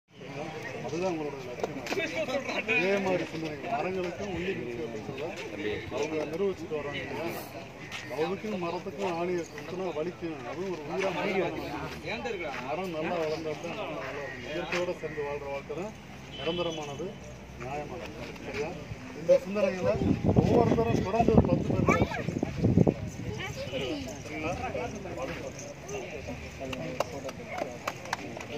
தேனியில்மரங்களில்ஆணிஎடுக்கும்போட்டி- தேனி காவல் ஆய்வாளர் முருகானந்தம் பாராட்டி பரிசு வழங்கினார்
Video from news photos saro